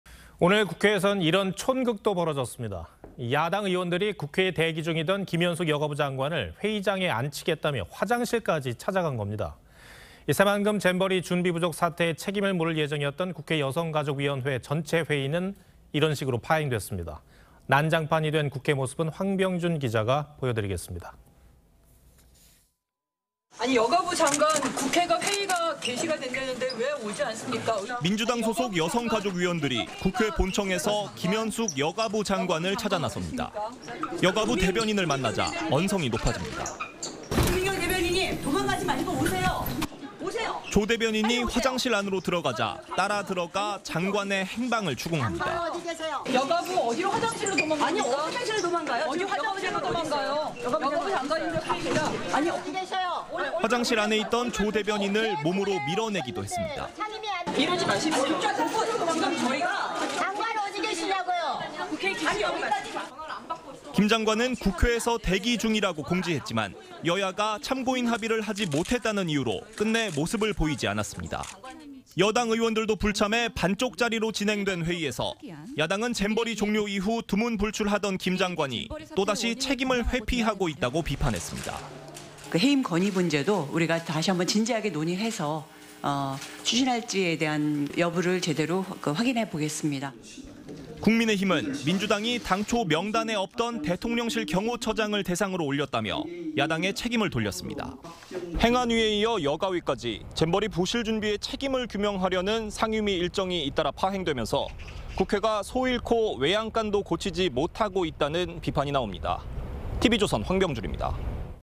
'잼버리 책임규명' 여가위, 김현숙 '잠수'에 파행
오늘 국회에선 이런 촌극도 벌어졌습니다. 야당 의원들이 국회에 대기 중이던 김현숙여가부 장관 을 회의장에 앉히겠다며 화장실까지 찾아간 겁니다. 새만금잼버리준비부족 사태의 책임을 물을 예정이었던 국회여성가족위원회전체회의 는 이런 식으로 파행됐습니다.